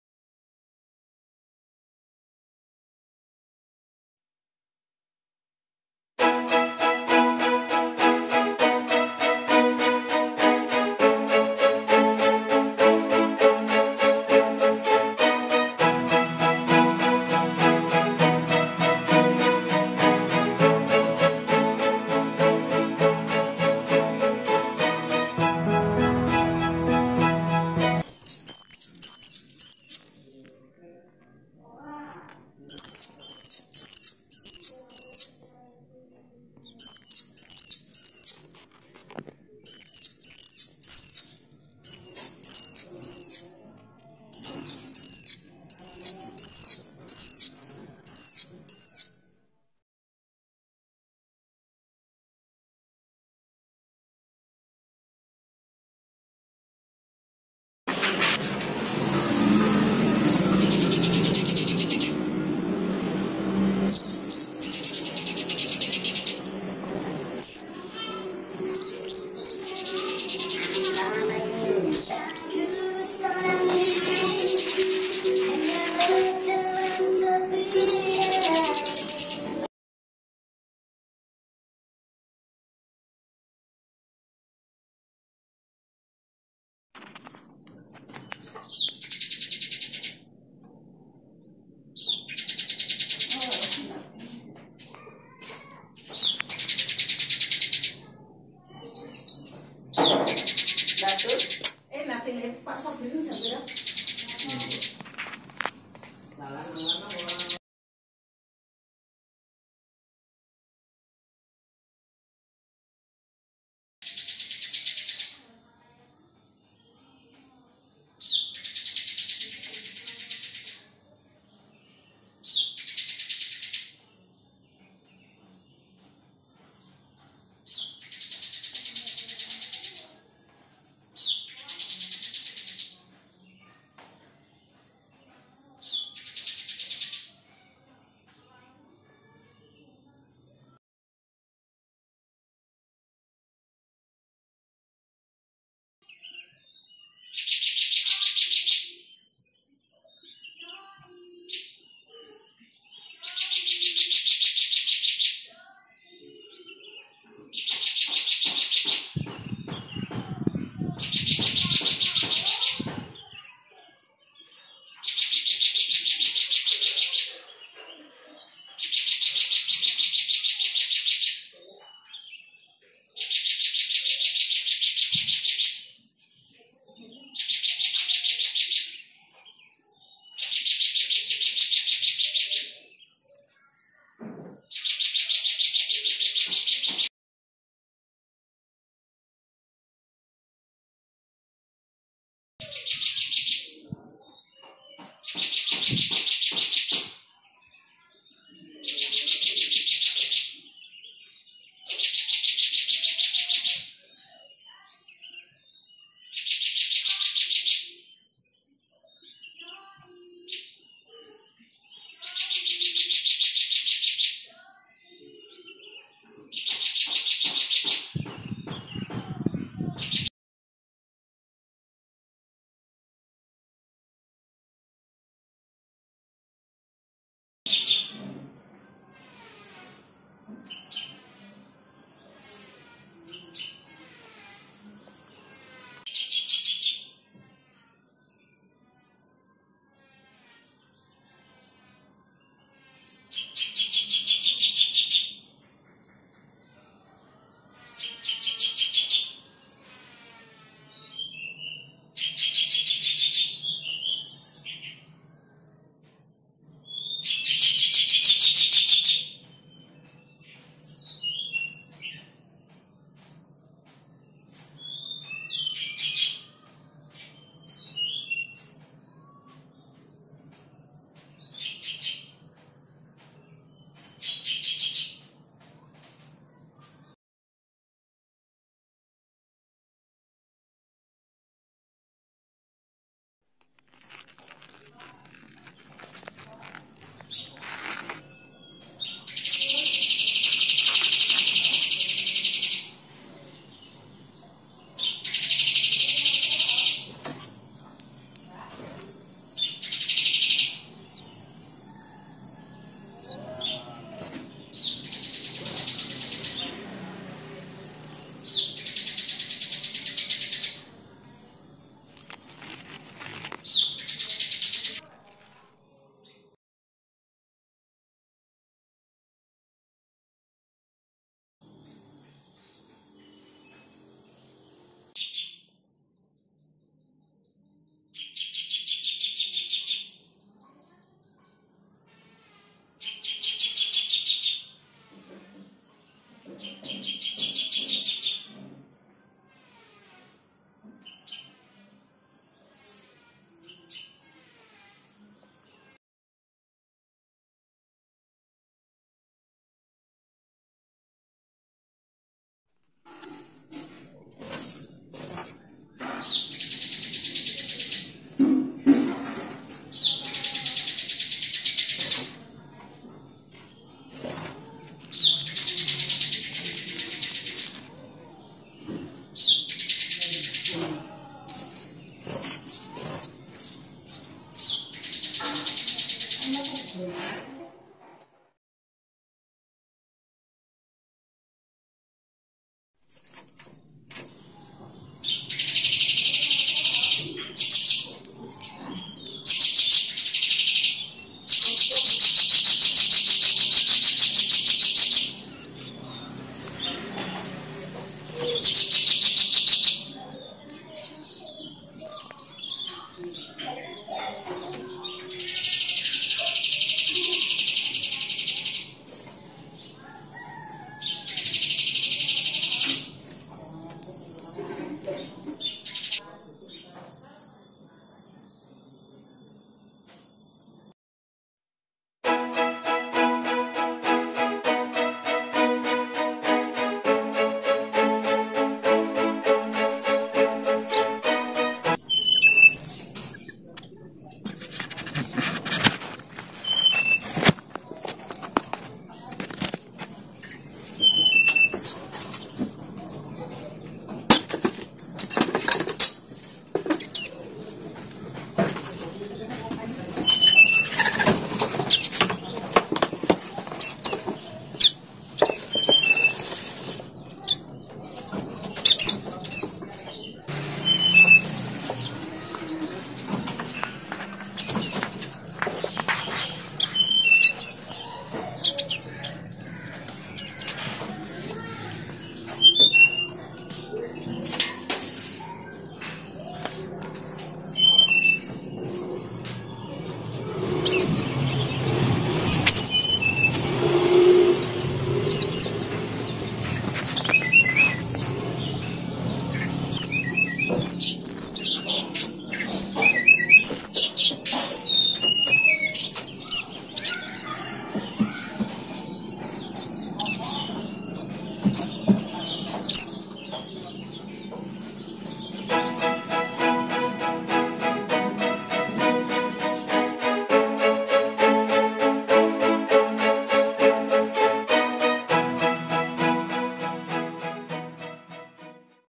Ternak Ciblek " ANAK CIBLEK UMUR 1 SAMPAI 12 HARI "
Video kali ini menayangkan tentang perkembangan anak ciblek dari mulai telur menetas sampai cindil burung berumur 12 hari disertai momen panen yang pasti di idam-idamkan setiap peternak dengan hasil yang memuaskan.
Pertumbuhan anak ciblek akan maksimal jika disertai dengan asupan gizi atau nutrisi pakan yang cukup dan akan menjadikan anakan ciblek tumbuh sehat sehingga terhindar dari rentan kematian pada anak burung yang masih diloloh.
Biasanya anak burung sudah bisa dipanen atau mulai diloloh sendiri oleh peternak kisaran umur 8 atau 9 hari seperti apa kira-kira besarnya bisa dilihat dalam video ini.

Semoga bermanfaat dan terimakasih.